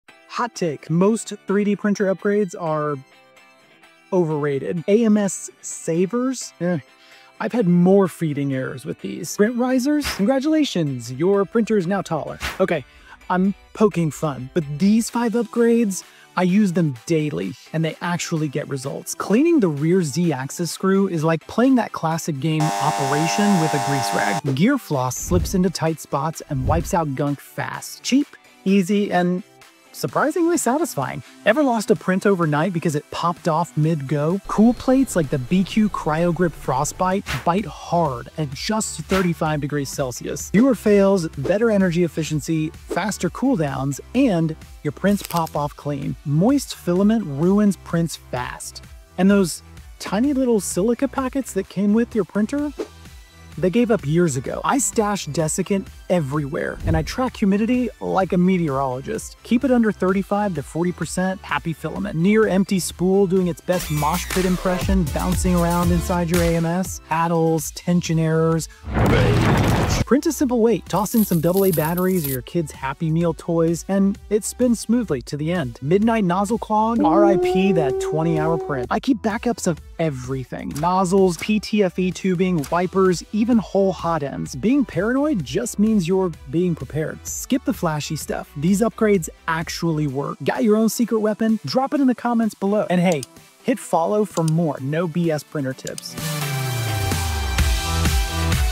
Most Printer Mods Are Useless. These Aren’t.
Most 3D printer mods look cool… but don’t actually help.
Here are 5 upgrades I actually use every day—real fixes that save prints, save time, and save your sanity.

✅ Better adhesion
✅ Cleaner movement
✅ Dryer filament
✅ Smoother spools
✅ Fewer fails

If you’re sick of useless mods that don’t improve print quality, this one’s for you.

👇 Drop your favorite “actually helpful” upgrade in the comments!